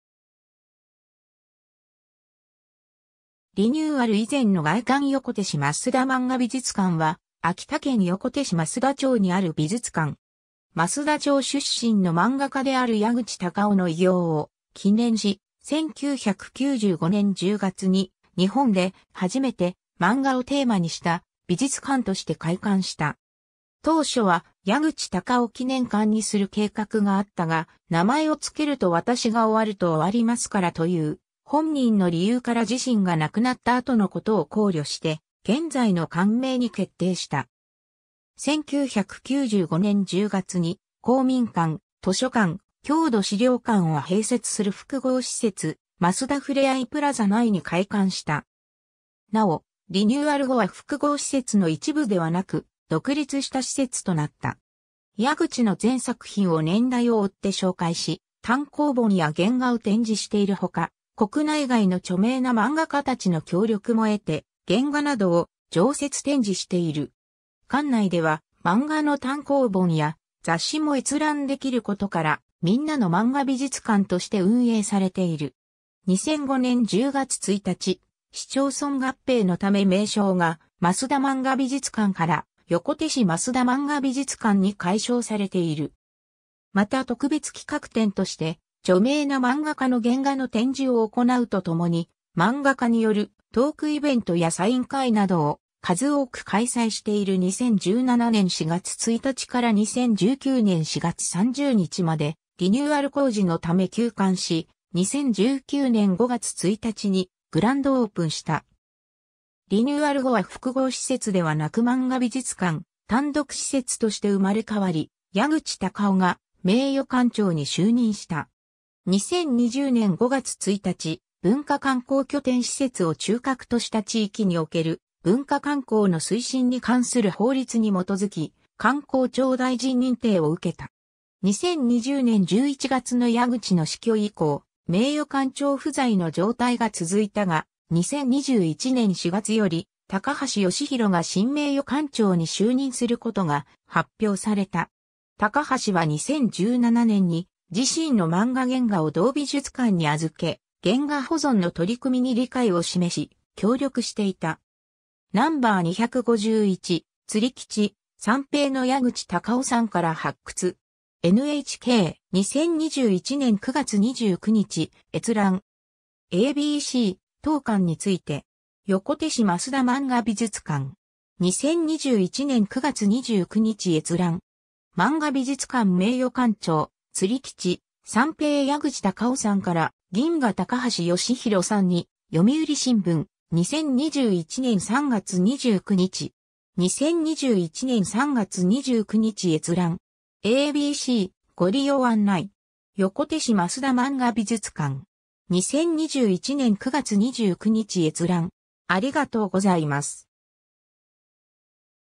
横手市増田まんが美術館
日本のマンガ・アニメ美術館とギャラリー
リニューアル以前の外観 横手市増田まんが美術館（よこてしますだまんがびじゅつかん）は、秋田県横手市増田町にある美術館。
増田町出身の漫画家である矢口高雄の偉業を記念し、1995年（平成7年）10月に日本で初めて「まんが」をテーマにした美術館として開館した。
 当初は『矢口高雄記念館』にする計画があったが、「名前を付けると私が終わると終わりますから」という本人の理由から自身が亡くなった後のことを考慮して現在の館名に決定した。
1995年10月に公民館、図書館、郷土資料館を併設する複合施設「増田ふれあいプラザ」内に開館した。
なお、リニューアル後は複合施設の一部ではなく、独立した施設となった。
 矢口の全作品を年代を追って紹介し単行本や原画を展示しているほか、国内外の著名な漫画家たちの協力も得て原画などを常設展示している。
館内では漫画の単行本や雑誌も閲覧できることから、「みんなのまんが美術館」として運営されている。
 2005年10月1日、市町村合併のため名称が「増田まんが美術館」から「横手市増田まんが美術館」に改称されている。
 また特別企画展として著名な漫画家の原画の展示を行うとともに、漫画家によるトークイベントやサイン会などを数多く開催している 2017年4月1日から2019年4月30日までリニューアル工事のため休館し、2019年5月1日にグランドオープンした。
リニューアル後は複合施設ではなくまんが美術館単独施設として生まれ変わり、矢口高雄が名誉館長に就任した。
 2020年5月1日、文化観光拠点施設を中核とした地域における文化観光の推進に関する法律（文化観光推進法）に基づき、観光庁大臣認定を受けた。
 2020年11月の矢口の死去以降、名誉館長不在の状態が続いたが、2021年4月より高橋よしひろが新名誉館長に就任することが発表された。
高橋は2017年に自身の漫画原画を同美術館に預け、原画保存の取り組みに理解を示し、協力していた。
 ^ “No.251「釣りキチ三平」の矢口高雄さんから発掘！”. NHK（日本放送協会） (2019年11月22日). 2021年9月29日閲覧。
 ^ a b c “当館について”. 横手市増田まんが美術館. 2021年9月29日閲覧。
 ^ 「「まんが美術館」名誉館長、「釣りキチ三平」矢口高雄さんから「銀牙」高橋よしひろさんに」『読売新聞』、2021年3月29日。
2021年3月29日閲覧。
 ^ a b c “ご利用案内”. 横手市増田まんが美術館. 2021年9月29日閲覧。